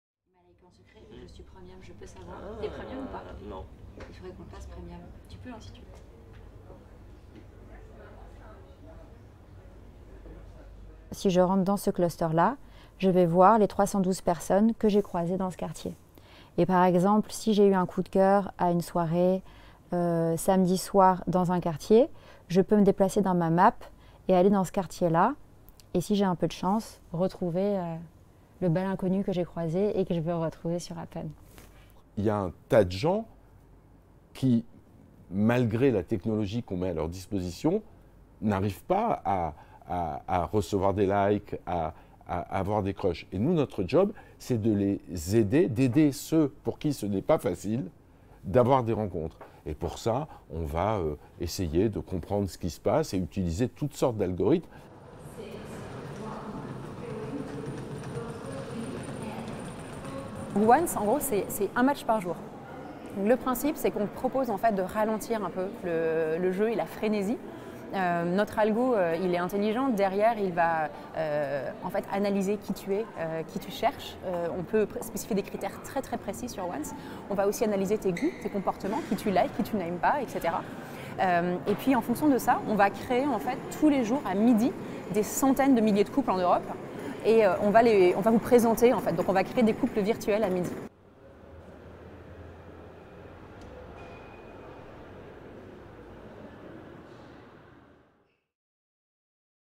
Valentine's Day: French dating apps face off against Anglo-saxon rivals | AFP
In the highly concentrated - and very lucrative - market of dating applications, two French companies are trying to make a difference. "Happn" works with geolocation to bring together strangers who have been in a nearby vicinity. "Once" on the other hand opts for "slow dating" by creating "virtual couples" every day at noon.